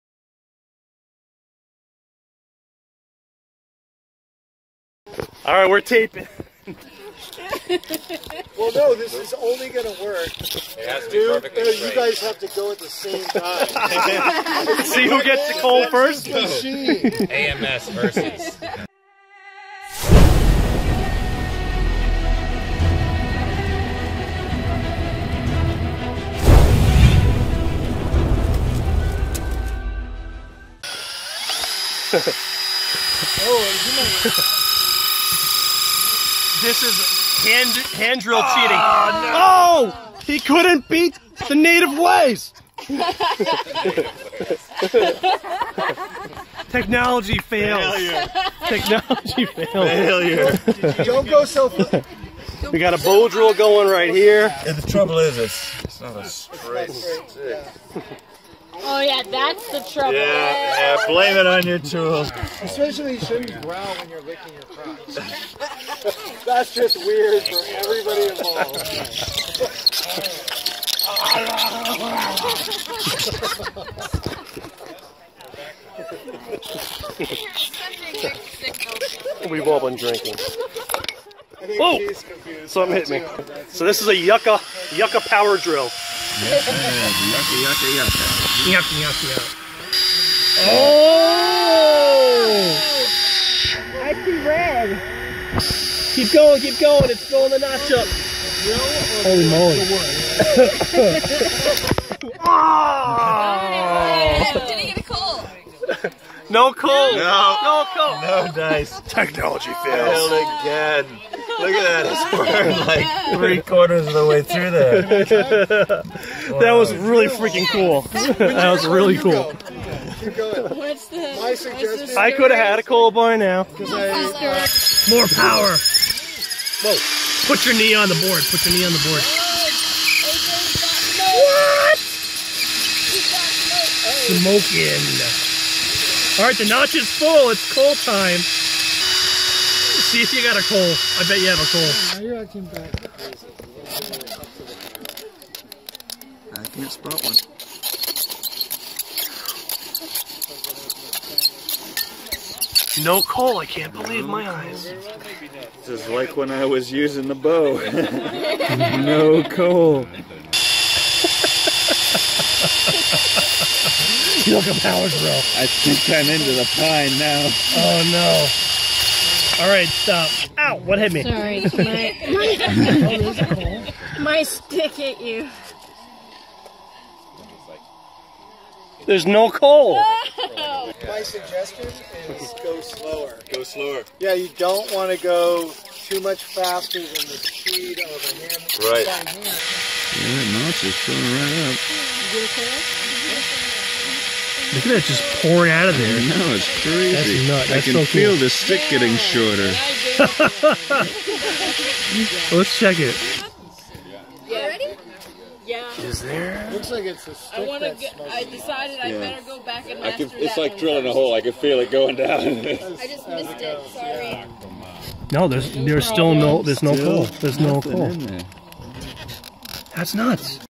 Not So Primitive Fire Making Experiment: YUCCA POWER DRILL!
This is what happens when a bunch of lunatics stay up all night deep in the woods of northern Vermont.  I had just done a bow drill fire making demo for the group and a handful of people had been trying to make a bow drill coal throughout the night.  One man got frustrated and tried to bring technology to the rescue! Nope.